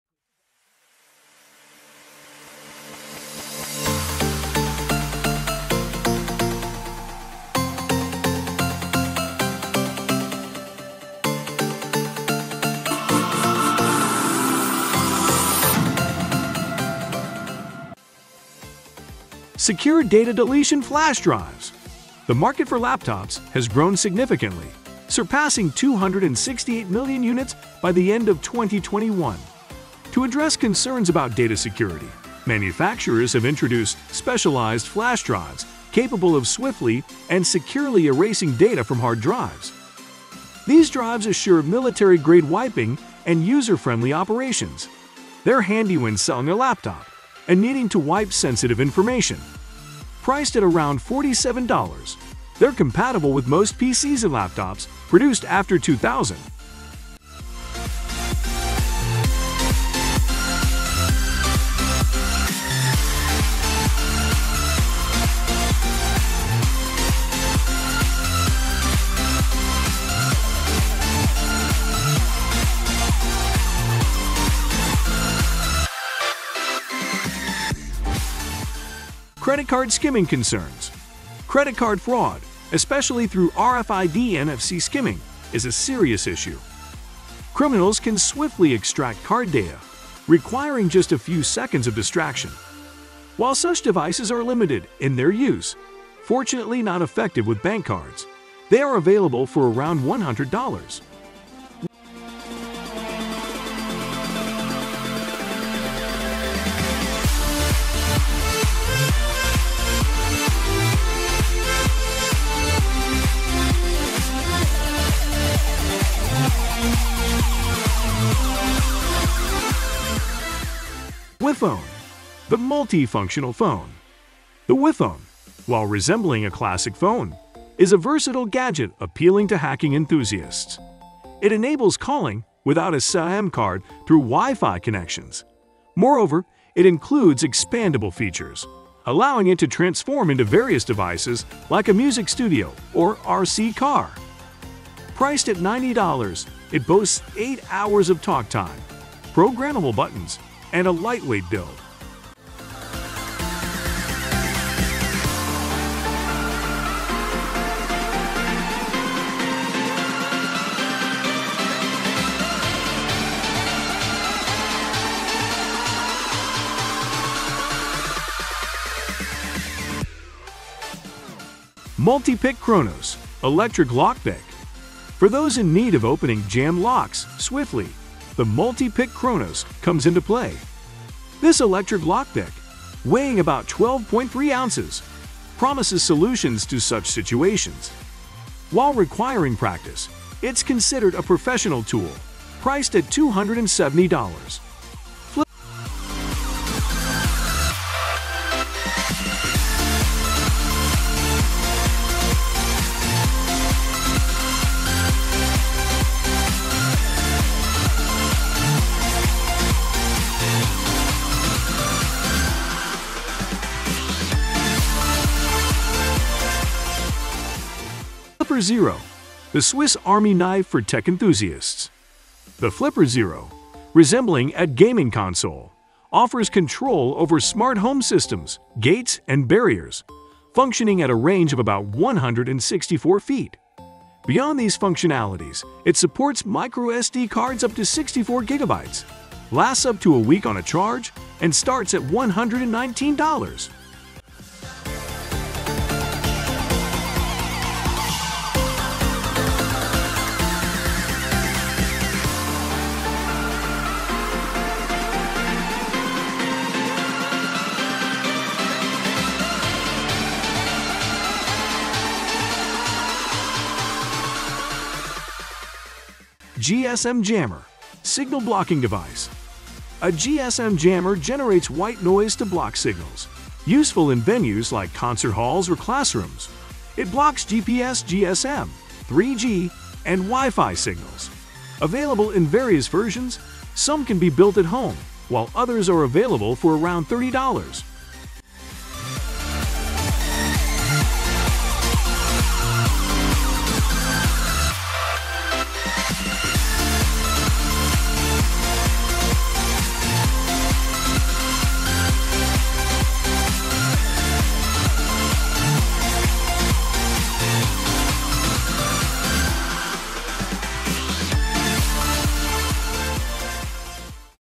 6 BANNED GADGETS YOU CAN STILL BUY ON AMAZON
0:00-Introduction
0:18-Secure Data Deletion Flash Drives
1:07-Credit Card Skimming Concerns
1:54-WiPhone
2:38-Multipick Kronos
3:24-Flipper Zero
4:16-GSM Jammer

💾 Secure Data Deletion Flash Drives: Say goodbye to worries about selling your laptop with sensitive info! These military-grade flash drives swiftly erase data for around $47, compatible with most PCs since 2000.

💳 Credit Card Skimming Concerns: Stay aware! Fraudsters use $100 gadgets for quick credit card data theft through RFID-NFC, but bank cards have some safety measures.

☎️ WiPhone: It's not just a phone! This $90 gem connects calls via Wi-Fi, perfect for hackers and tech fans. Plus, it transforms into cool stuff like a music studio or RC car!

🔑 Multipick Kronos: Unlock jammed locks like a pro! This 12.3-ounce electric lock pick at $270 needs practice but gets you out of tight spots fast.

🎮 Flipper Zero: The ultimate tech Swiss Army knife! Control smart systems within 164 feet, store up to 64GB on micro SD, and it lasts a week on a charge. Starting at $119, it's a game-changer!

🚫 GSM Jammer: Silence signals with white noise! Perfect for venues like concert halls or classrooms. Some cost around $30 and block GPS, GSM, 3G, and Wi-Fi signals.